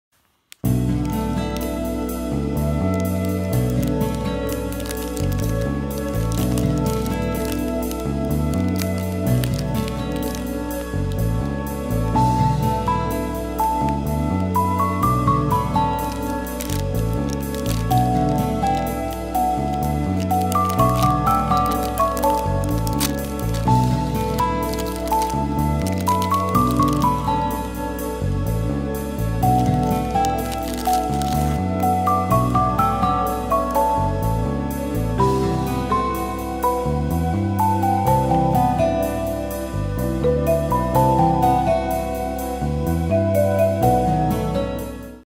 【スイーツ（JAPAN sweets）】チョコバッキー バニラ味「シャトレーゼ（Chateraise）」
今大人気のチョコバッキー
いつもはスカイベリーを食べていますが久々にバニラ味を食べました。
やっぱりチョコバッキーは旨いねっ

【チョコバッキー（バニラ味）】（スイーツ　JAPAN sweets）